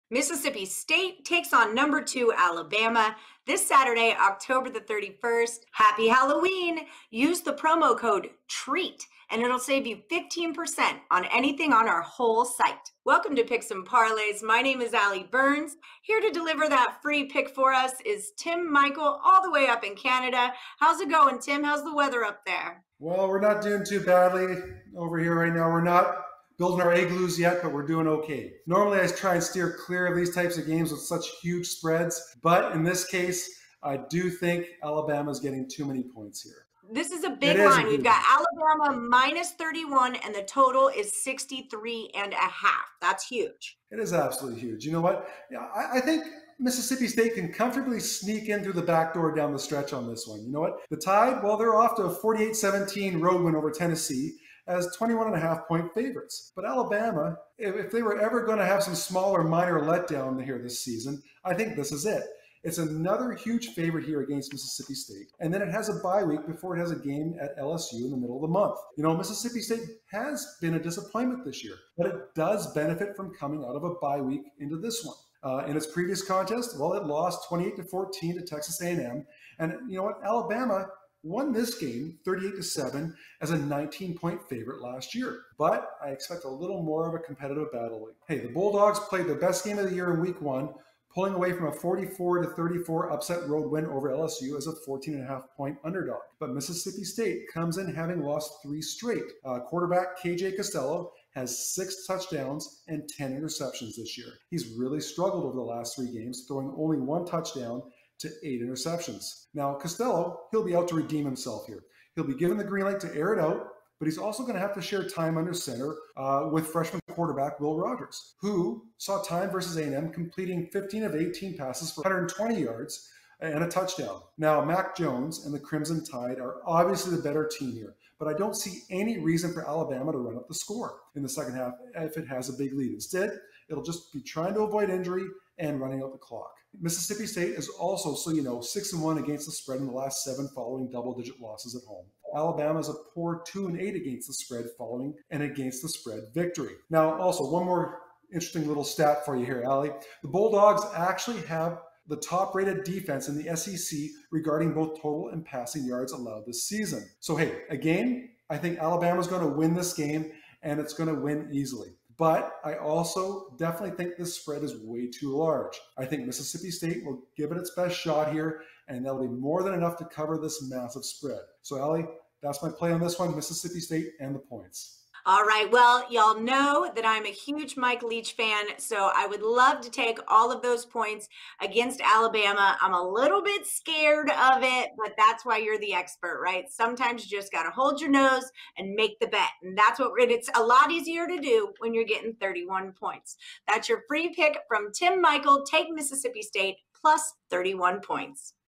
Mississippi State at Alabama - Saturday 10/31/20 - NCAAF Picks & Predictions l Picks & Parlays
Mississippi State Bulldogs at Alabama Crimson Tide - Saturday 10/31/20 - College Football Picks & Predictions

College Football moves on and we have all your betting needs covered. Picks & Parlays experts Ali Burns and Tim Michael has the Free Betting Pick and Prediction in this Mississippi State Bulldogs at Alabama Crimson Tide matchup for Saturday October 31, 2020.